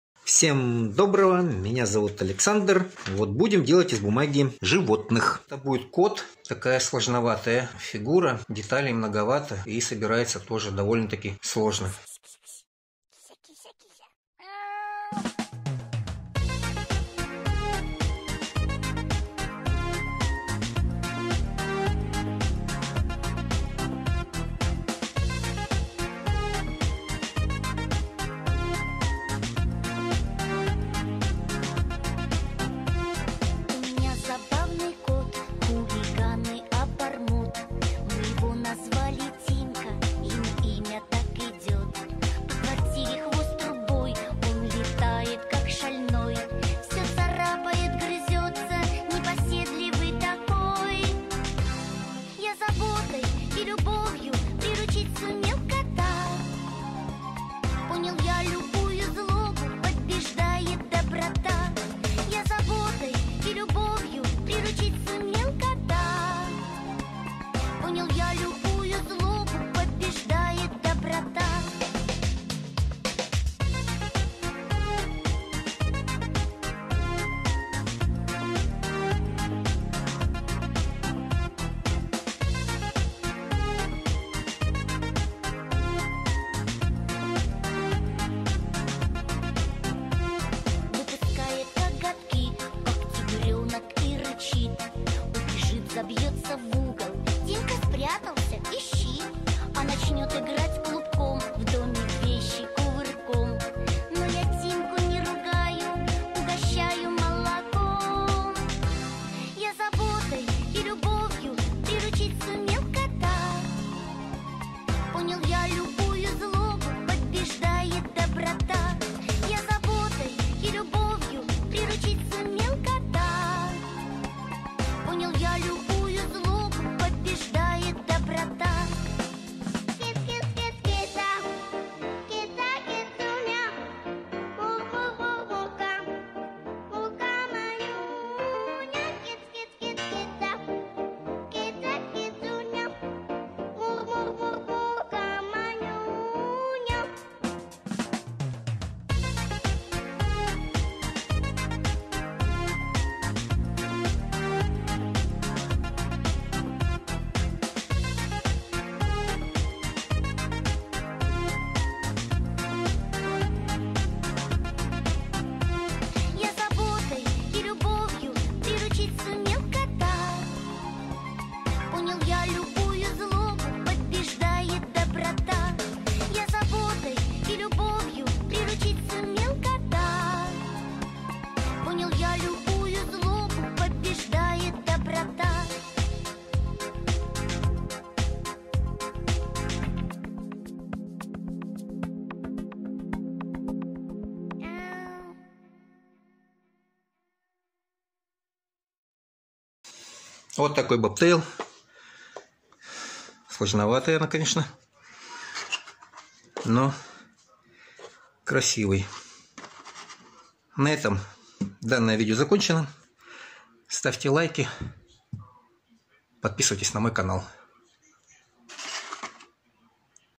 Кот из бумаги. Белый бобтейл.
Довольно сложная фигурка. Много деталей, сравнительно большие размеры. Сложность заключается в том числе и в том, что фигурка бескаркасная - так называемая "скорлупа". Но если сделать аккуратно, то результат впечатляет.